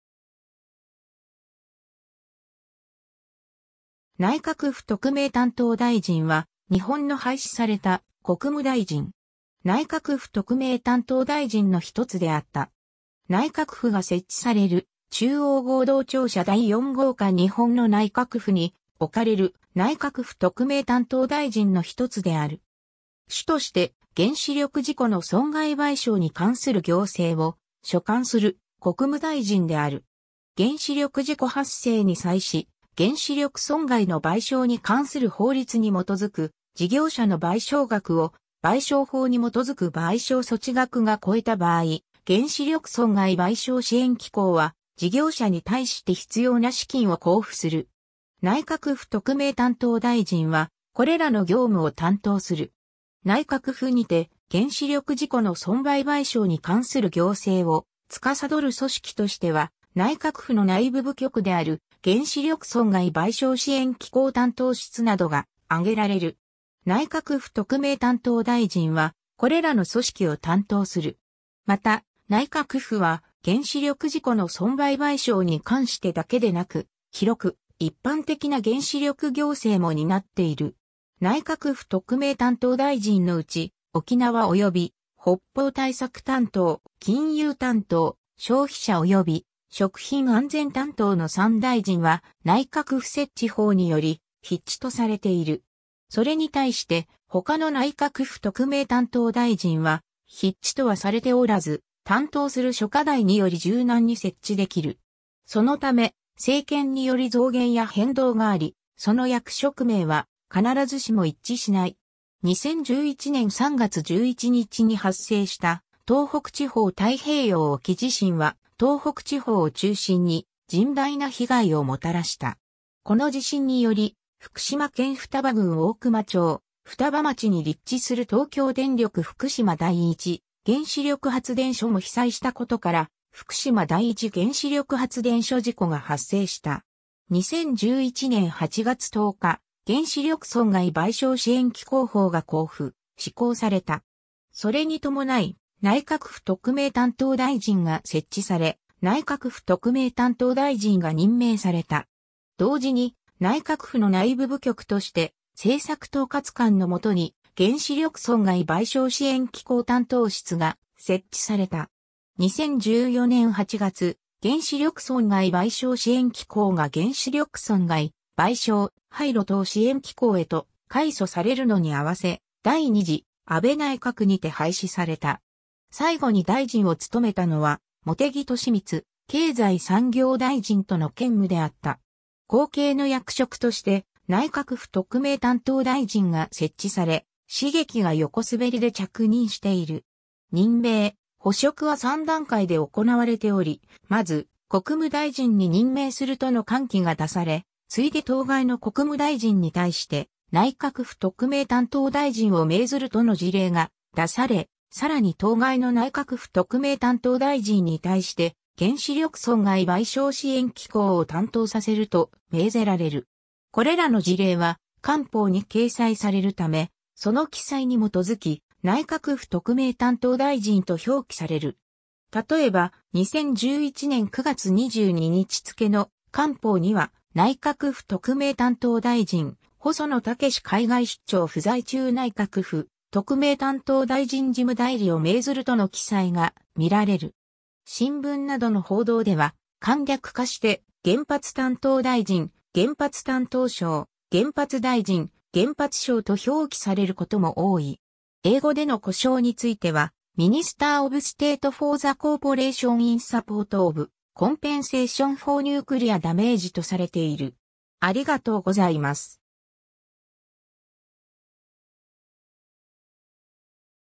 内閣府特命担当大臣（原子力損害賠償支援機構担当）
内閣府特命担当大臣_(廃止)
内閣府特命担当大臣（原子力損害賠償支援機構担当）（ないかくふとくめいたんとうだいじん げんしりょくそんがいばいしょうしえんきこうたんとう、英語: Minister of State for the Corporation in Support of Compensation for Nuclear Damage）は、日本の廃止された国務大臣。
内閣府特命担当大臣の一つであった。
 内閣府（科学技術政策・イノベーション担当）が設置される中央合同庁舎第四号館 日本の内閣府に置かれる内閣府特命担当大臣の一つである。
主として原子力事故の損害賠償に関する行政を所管する国務大臣である。
 原子力事故発生に際し、原子力損害の賠償に関する法律に基づく事業者の賠償額を賠償法に基づく賠償措置額が超えた場合、原子力損害賠償支援機構は事業者に対して必要な資金を交付する。
内閣府特命担当大臣（原子力損害賠償支援機構担当）は、これらの業務を担当する。
 内閣府にて原子力事故の損賠賠償に関する行政を司る組織としては、内閣府の内部部局である原子力損害賠償支援機構担当室などが挙げられる。
内閣府特命担当大臣（原子力損害賠償支援機構担当）は、これらの組織を担当する。
また、内閣府は、原子力事故の損賠賠償に関してだけでなく、広く一般的な原子力行政も担っている。
 内閣府特命担当大臣のうち、沖縄及び北方対策担当、金融担当、消費者及び食品安全担当の3大臣は、内閣府設置法により必置とされている。
それに対して、他の内閣府特命担当大臣は必置とはされておらず、担当する諸課題により柔軟に設置できる。
そのため、政権により増減や変動があり、その役職名は必ずしも一致しない。
 2011年3月11日に発生した東北地方太平洋沖地震は、東北地方を中心に甚大な被害をもたらした。
この地震により福島県双葉郡大熊町・双葉町に立地する東京電力福島第一原子力発電所も被災したことから、福島第一原子力発電所事故が発生した。
 2011年8月10日、原子力損害賠償支援機構法が公布・施行された。
それにともない、内閣府特命担当大臣（原子力損害賠償支援機構担当）が設置され、内閣府特命担当大臣が任命された。
同時に、内閣府の内部部局として、政策統括官（科学技術政策・イノベーション担当）の下に原子力損害賠償支援機構担当室が設置された。
2014年8月、原子力損害賠償支援機構が原子力損害賠償・廃炉等支援機構へと改組されるのに合わせ、第2次安倍内閣にて廃止された。
最後に大臣を務めたのは茂木敏充、経済産業大臣との兼務であった。
後継の役職として内閣府特命担当大臣（原子力損害賠償・廃炉等支援機構担当）が設置され、茂木が横滑りで着任している。
 任命、補職は3段階で行われており、まず「国務大臣に任命する」との官記が出され、次いで当該の国務大臣に対して「内閣府特命担当大臣を命ずる」との辞令が出され、さらに当該の内閣府特命担当大臣に対して「原子力損害賠償支援機構を担当させる」と命ぜられる。
これらの辞令は『官報』に掲載されるため、その記載に基づき「内閣府特命担当大臣（原子力損害賠償支援機構担当）」と表記される。
たとえば、2011年9月22日付の『官報』には「内閣府特命担当大臣細野豪志海外出張不在中内閣府特命担当大臣（原子力損害賠償支援機構担当）事務代理を命ずる」との記載が見られる。
新聞などの報道では、簡略化して「原発担当大臣」「原発担当相」「原発大臣」「原発相」と表記されることも多い。
英語での呼称については「Minister of State for the Corporation in support of Compensation for Nuclear Damage」とされている。